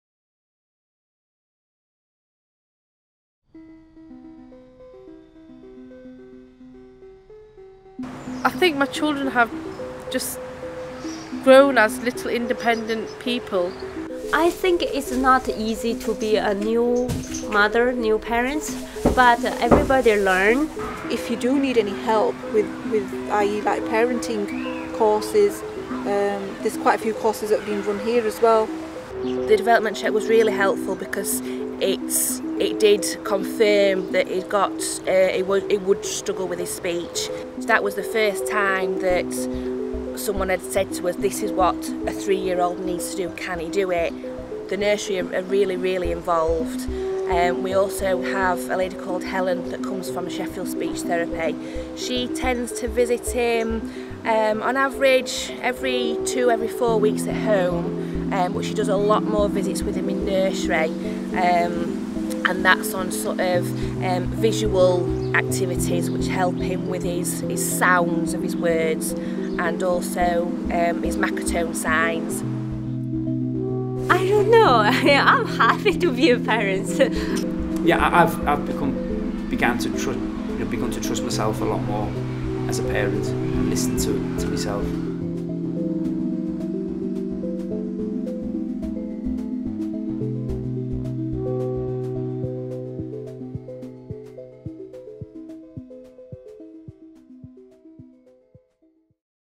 Families in the Foundation Years: Encouraging independence
Families in the Foundation Years sets out the Government's vision for our offer to parents, from pregnancy to age 5. View this clip to hear experiences of parenting children in their earliest years.